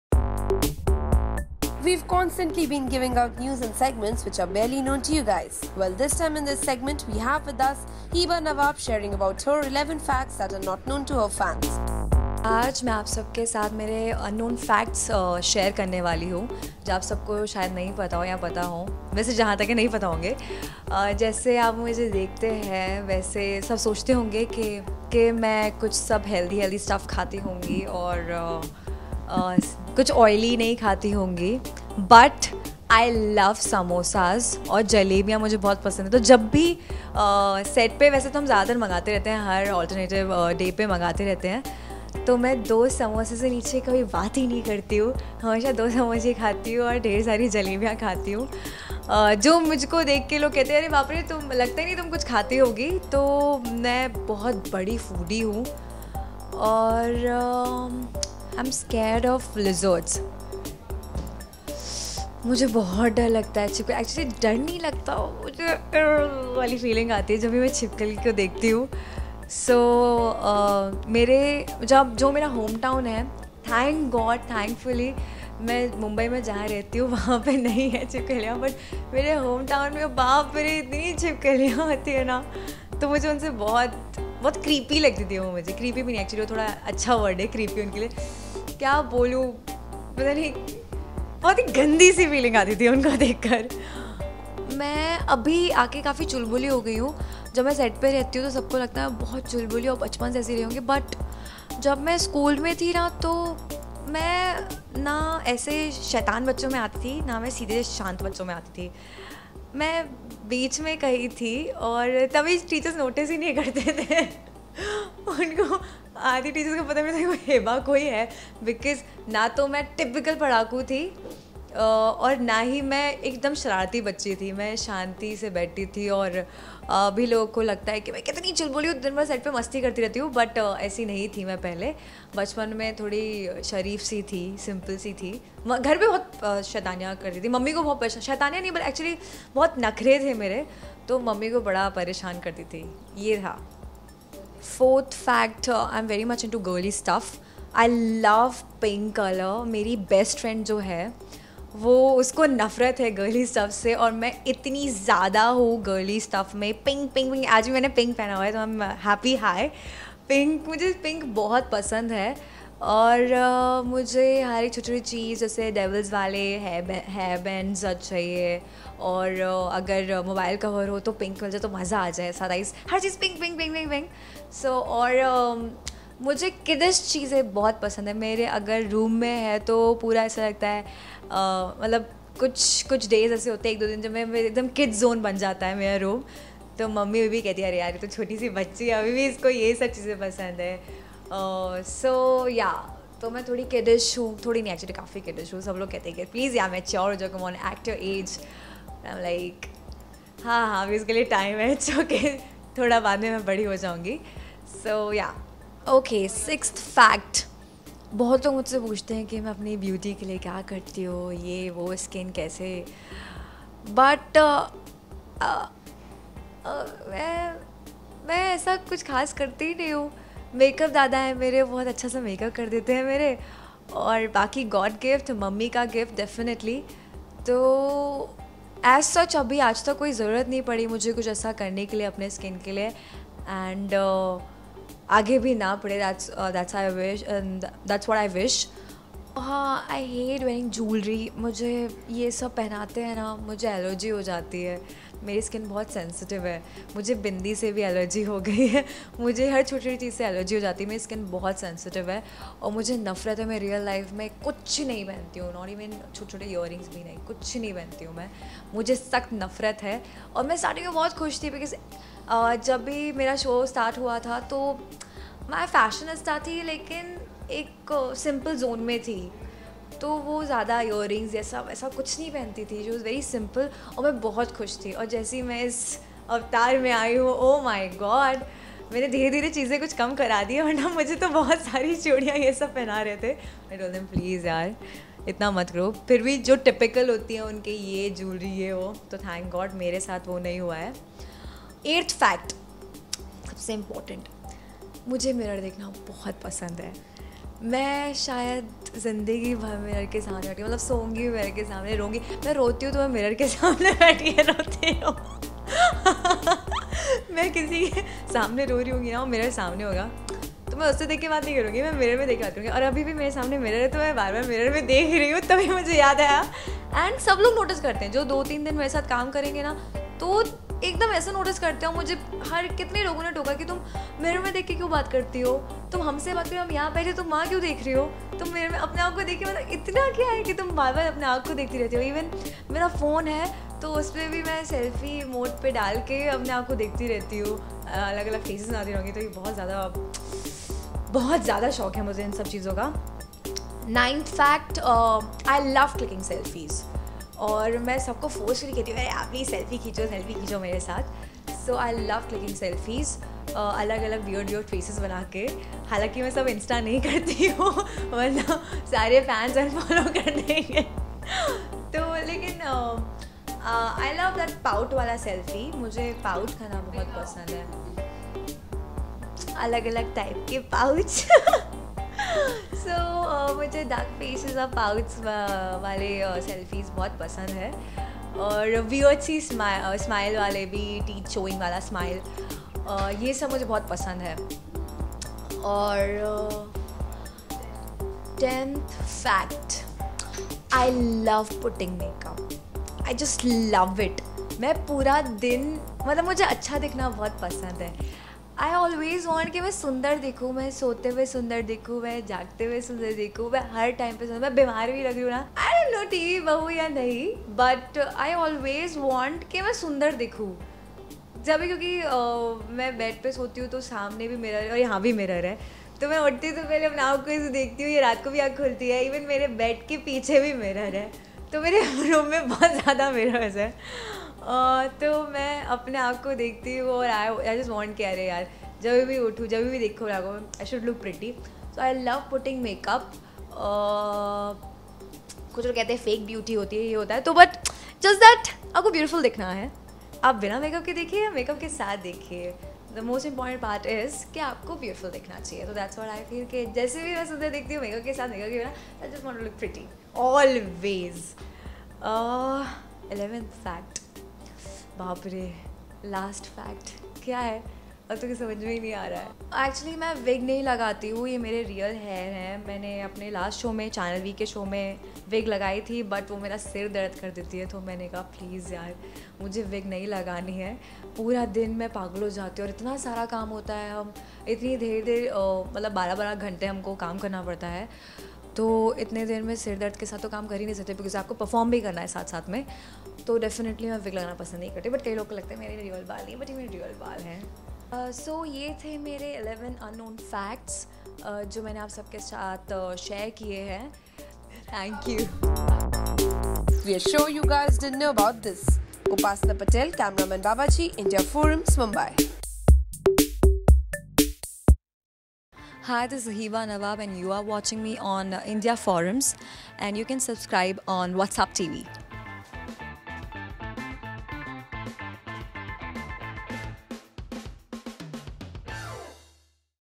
Hiba Nawab shares her 11 not known facts
Hiba Nawab from Star Plus's Tere Sheher Mein speaks about her 11 unknown facts. Hit the play button and check out